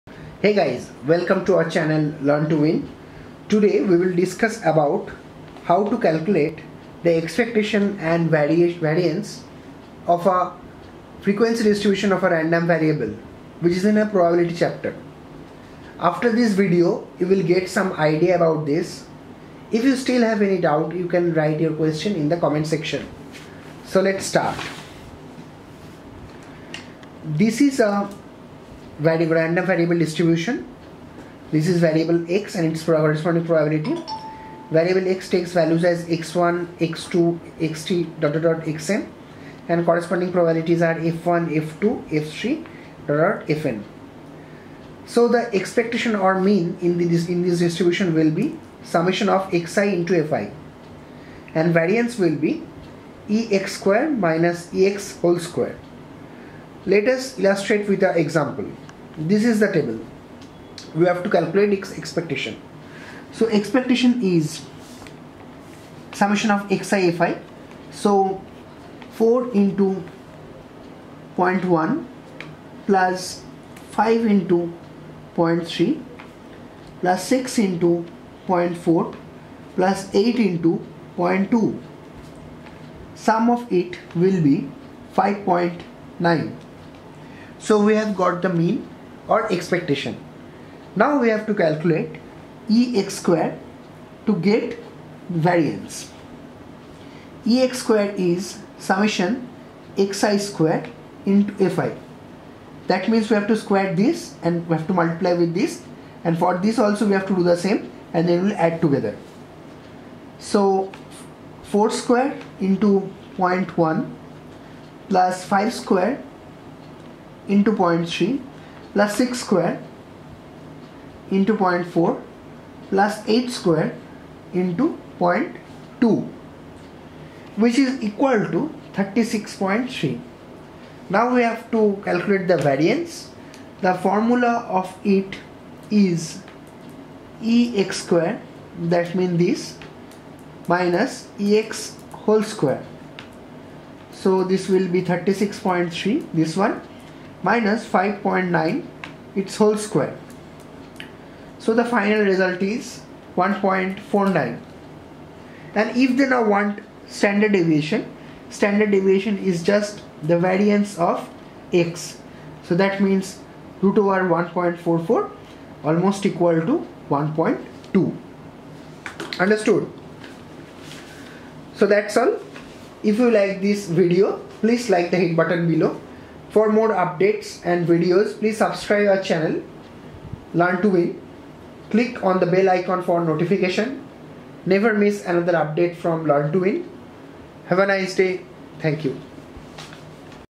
How to calculate Expectation & Variance of a random variable: Mathematics (CBSE, ISC)
How to calculate Expectation and Variance of a random variable of any probability distribution: Class XII Mathematics